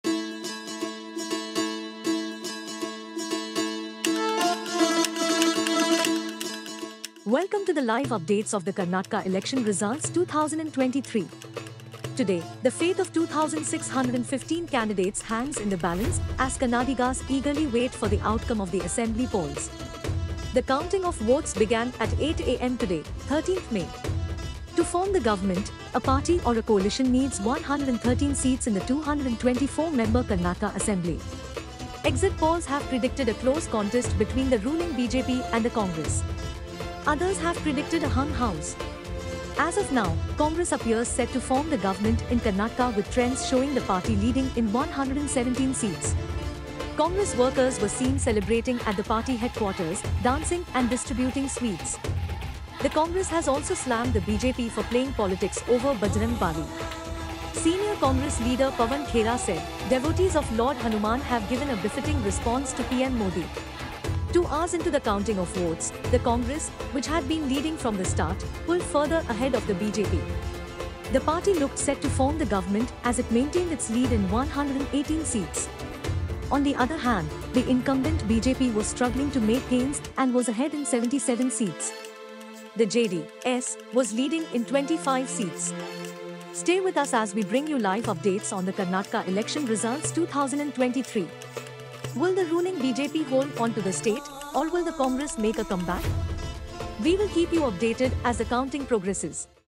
Congress or BJP? Who will Karnataka choose? Find out LIVE now
Watch the live updates of Karnataka Election Results 2023, as Kannadigas eagerly wait for the outcome of the assembly polls. With the Congress leading in 118 seats, will the ruling BJP hold on to the state, or will the Congress make a comeback?